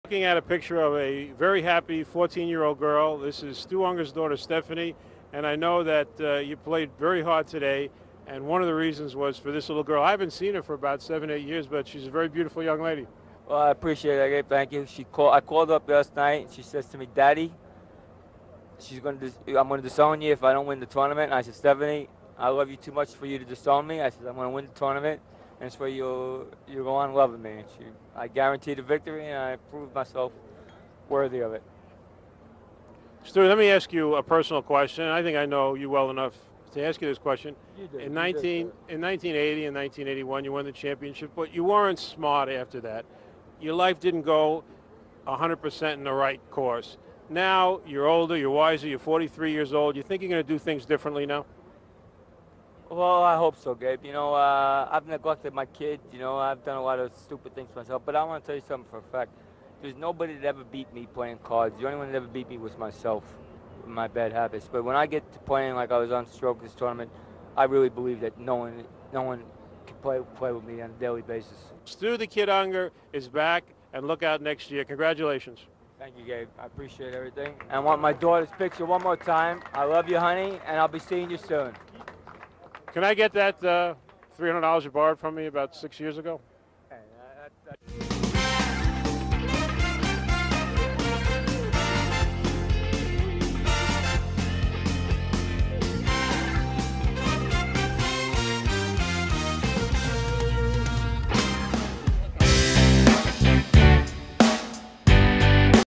Last Interview of Stu Ungar
After winning the 1997 World Series of Poker, Stu Ungar is interviewed by Gabe Kaplan in what would be Stu's last in the spotlight interview. This was Stu Ungar’s third time of winning the WSOP, cementing him into Poker Immortality. Stu also  won Amarillo Slim's Super Bowl of Poker three times.